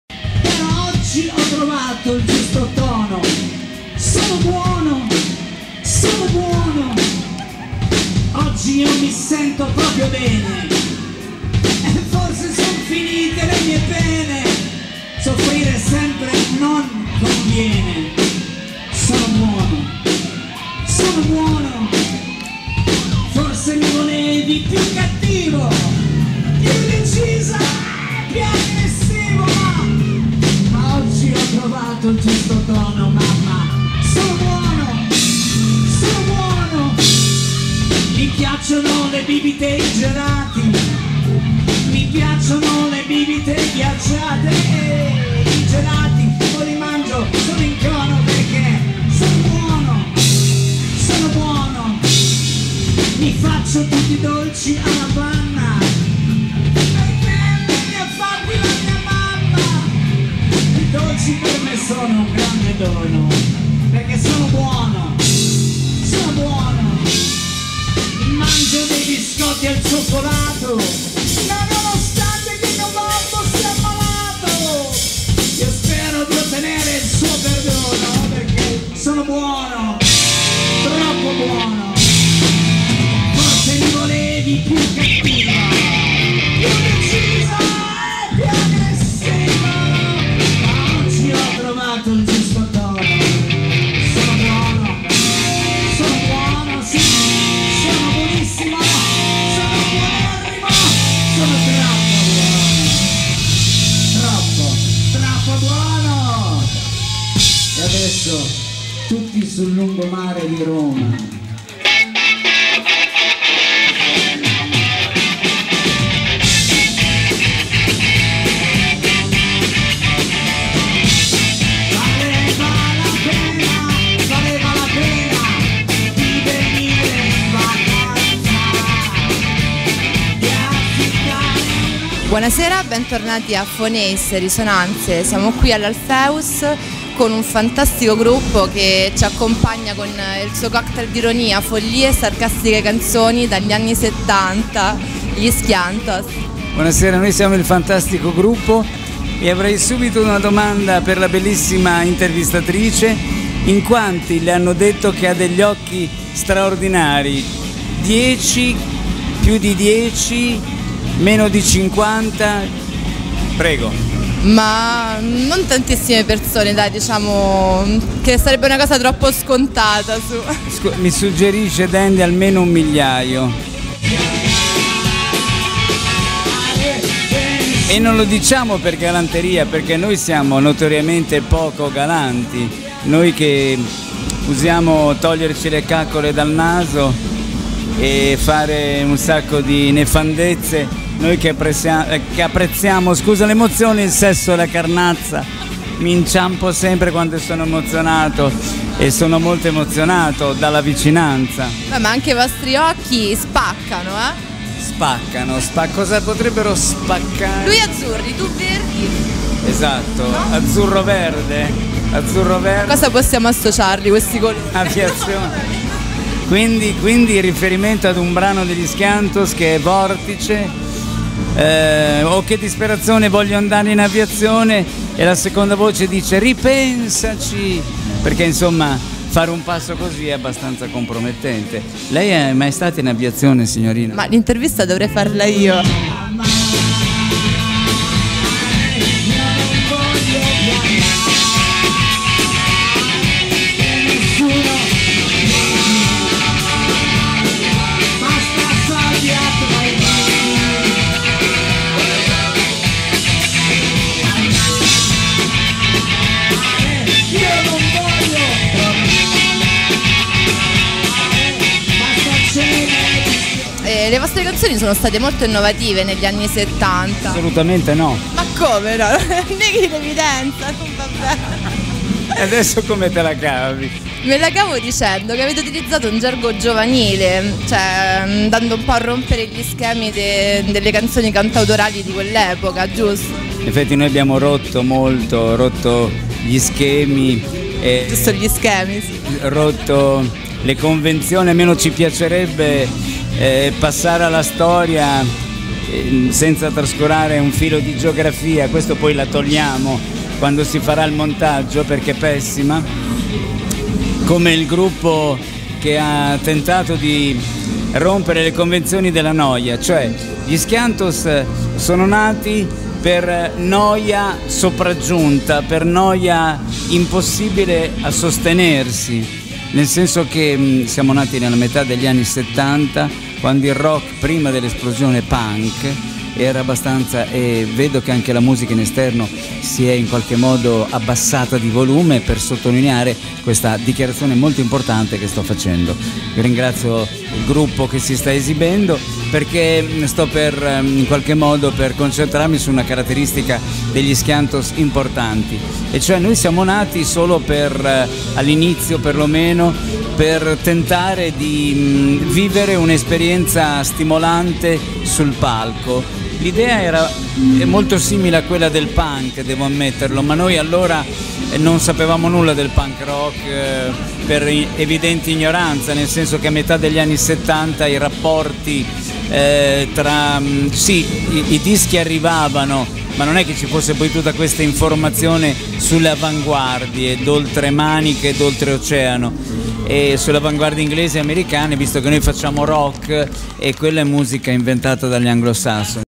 SKIANTOS Parte1.avi
Il gruppo bolognese, riconosciuto ormai all' unanimità come fondatore del genere rock demenziale, suona all' Alpheus riproponendo nel repertorio brani classici a fianco delle nuove produzioni. Attivi dal 1975, hanno contribuito in maniera notevole a creare il movimento culturale anticonformista e di rottura che tanto ha avuto seguito tra i giovani del periodo. Tutta la voglia degli Skiantos di proporre nel corso degli anni idee innovative, almeno per il pubblico italiano, viene raccontata nell' intervista di questo speciale dai componenti storici del gruppo.